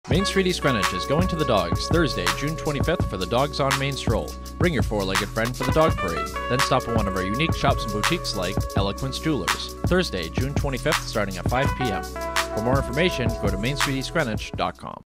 Dogs on Main Stroll - June 25 @ 5PM - Eloquence Jewelers
Main Street East Greenwich will be going to the dogs Thursday June 25th for the “Dogs on Main Stroll”! The Stroll starts at 5PM with the famous dog parade set begin around 6PM. For those wishing to participate in the parade there is no registration necessary.  Paraders are asked to come to 500 Main Street in front of Back to Basics for a barbecue sponsored by Hill & Harbour Veterinary Center and to gather before the parade starts. Perfect Pooch will also have a free pet photography station set up as well so make sure your pooch is looking his or her best!

The parade will leave the Back to Basics parking lot and head north on Main Street and end at Town Hall. Prizes will then be awarded for the best pet costumes and best owner costumes by the leader of the event Bone Appetit!

Then stay on Main Street and make sure to stop in some shops and restaurants and support the businesses that make this great event possible.

We hope everyone has a great night of shopping, food, friends, family and fun!

For questions regarding the Stroll call the EG Chamber at 885-0020.